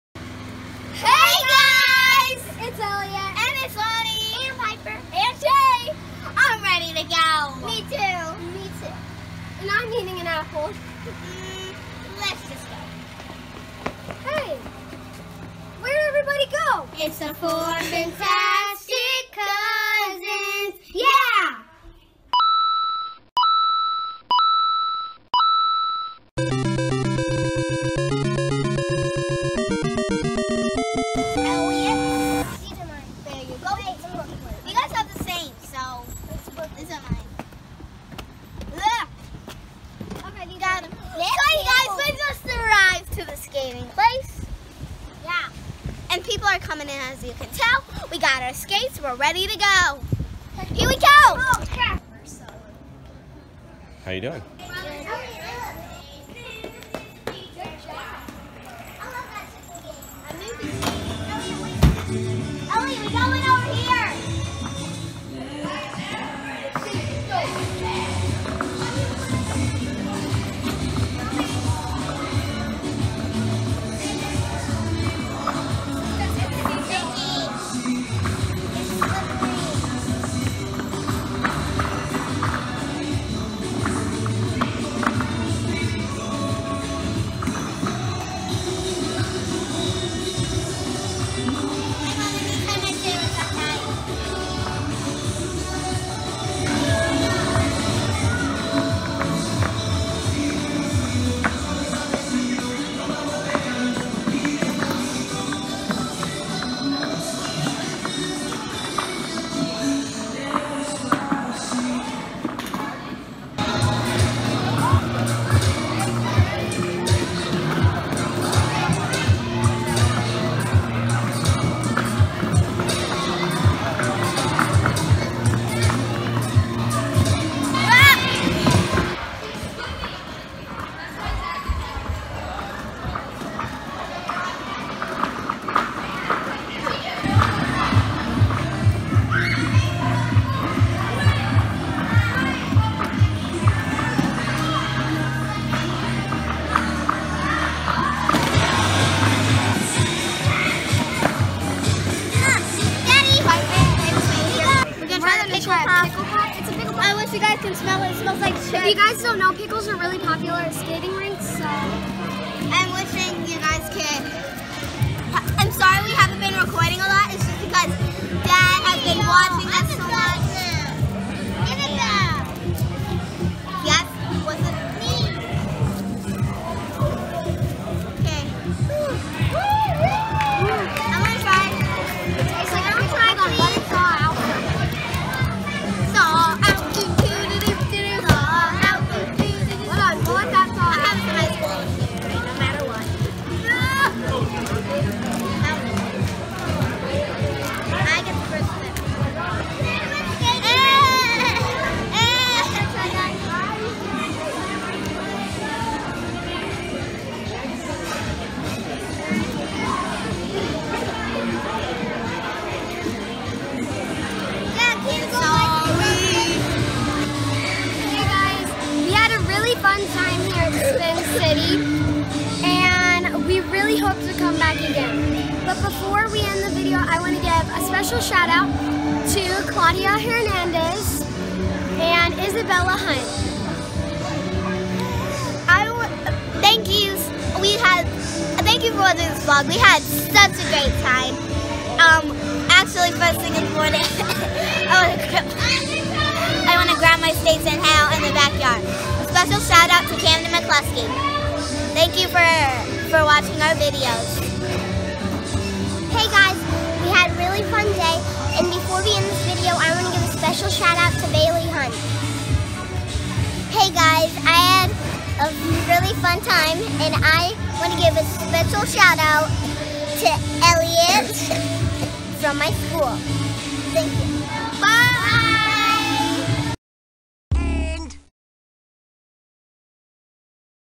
Spin City Skating (Vlog #3)
Today, we went for a visit at Spin City Skating in Troy, IL